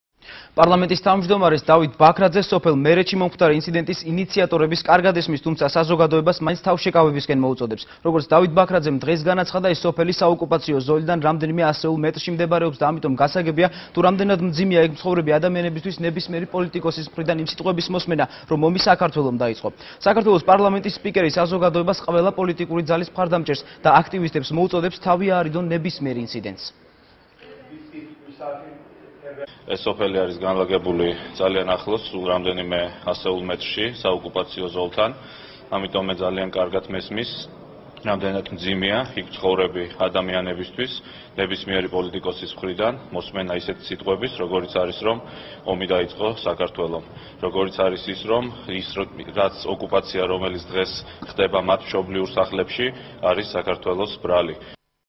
ახალი 6 | ბაქრაძის კომენტარი | 26.06.12
პარლამენტის თავმჯდომარეს დავით ბაქრაძეს "სოფელ მერეთში მომხდარი ინციდენტის ინიციატორების კარგად ესმის", თუმცა საზოგადოებას მაინც თავშეკავებისკენ მოუწოდებს. როგორც დავით ბაქრაძემ დღეს განაცხადა, ეს სოფელი საოკუპაციო ზოლიდან რამდენიმე ასეულ მეტრში მდებარეობს და ამიტომ, გასაგებია, თუ რამდენად მძიმეა იქ მცხოვრები ადამიანებისთვის ნებისმიერი პოლიტიკოსის მხრიდან იმ სიტყვების მოსმენა, რომ ომი საქრთველომ დაიწყო.  საქართველოს პარლამენტის სპიკერი საოზგადეობას, ყველა პოლიტიკური ძალის მხარდამჭერს და აქტივისტს მოუწოდებს, რომ თავი აარიდონ ნებისმიერ ინციდენტს.